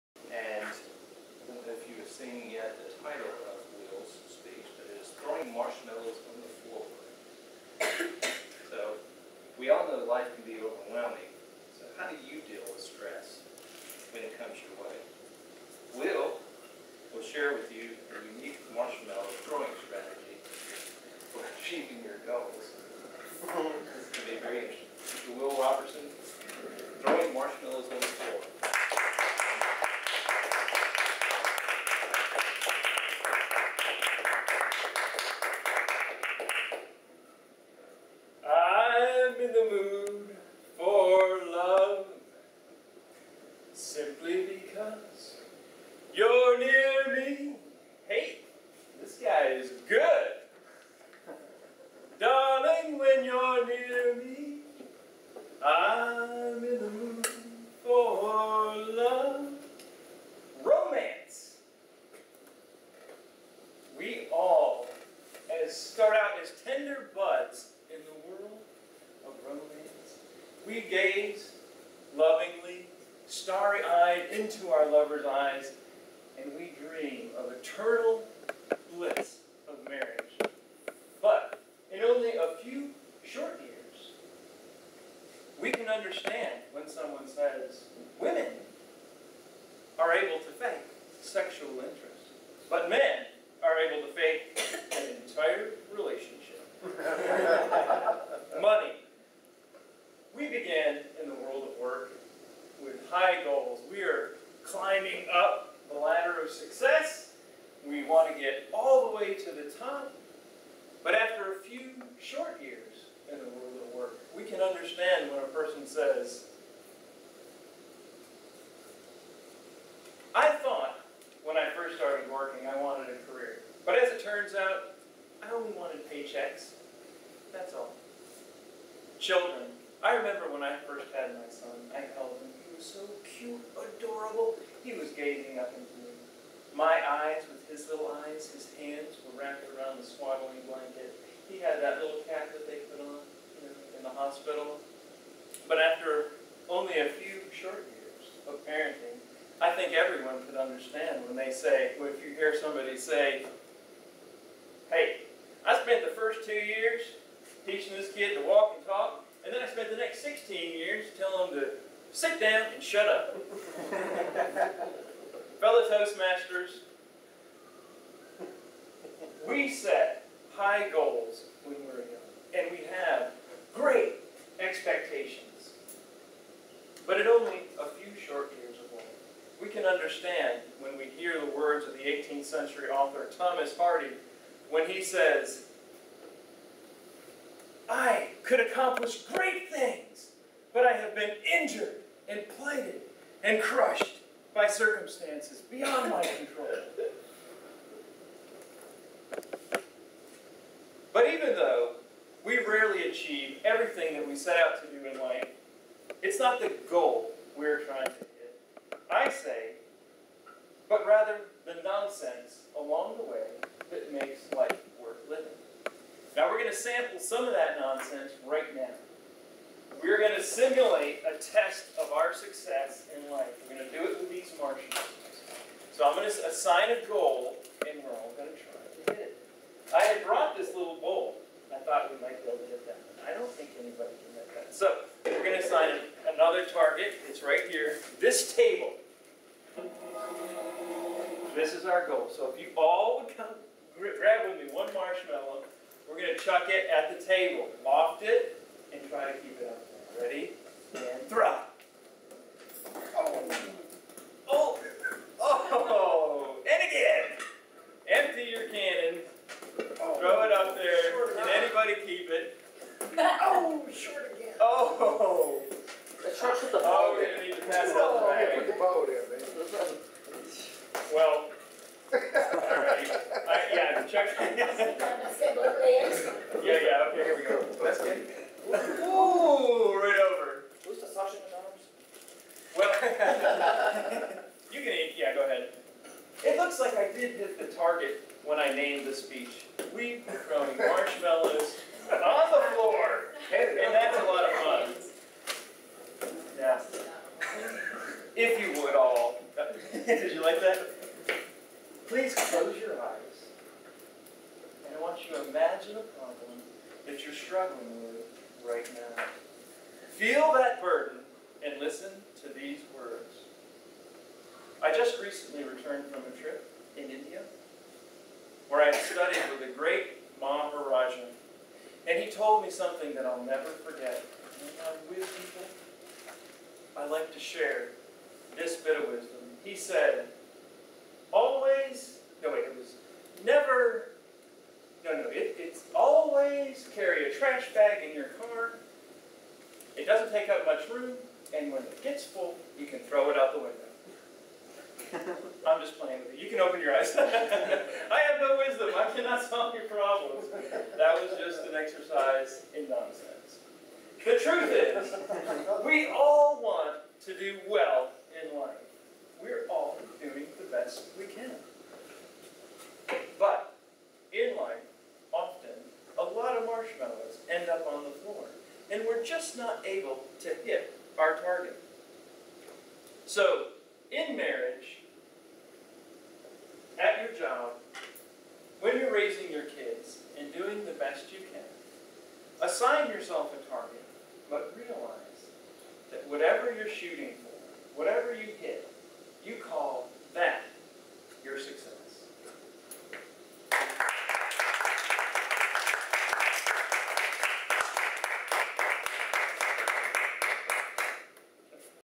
Throwing Marshmellows on the Floor - Will Robertson, 09/17/2012
Humorously Speaking, Project 4: Keep Them Laughing